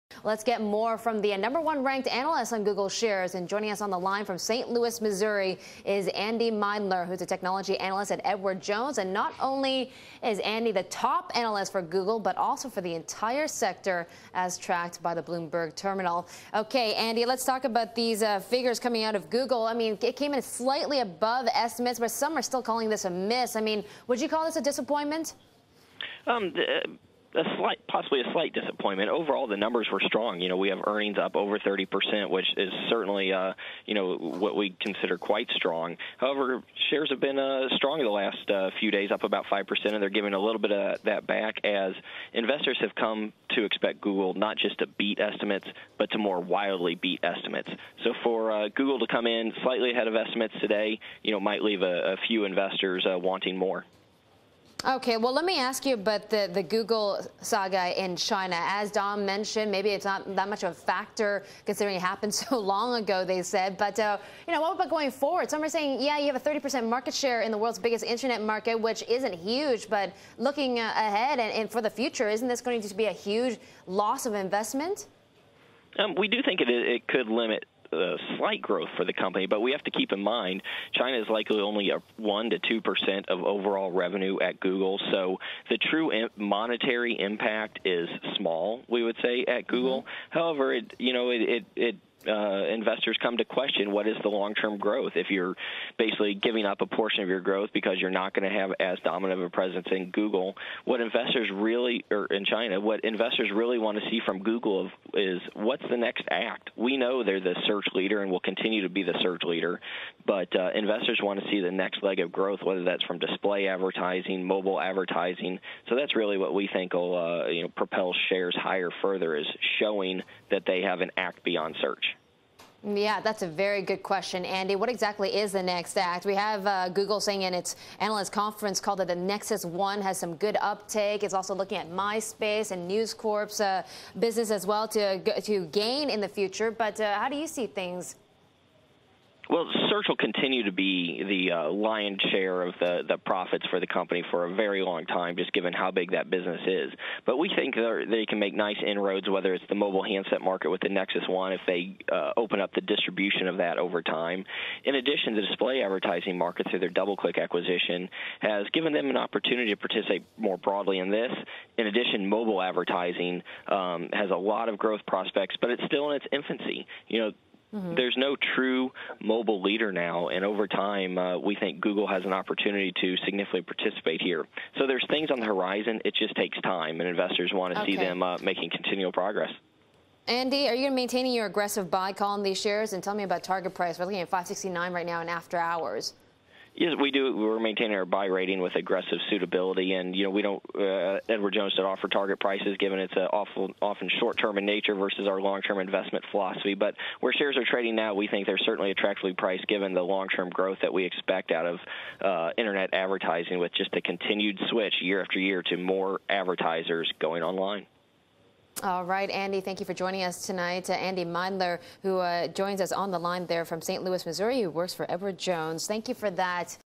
Analyst Miedler Says Google Results `Quite Strong': Video
April 16 (Bloomberg) -- Andy Miedler, an analyst with Edward Jones & Co. in St. Louis., talks with Bloomberg's Susan Li about Google Inc.'s financial results and growth outlook.
     Google Inc., owner of the worlds most popular search engine, reported profit that fell short of some analysts estimates, a sign that online advertising may not be rebounding as fast as some had anticipated. (Source: Bloomberg)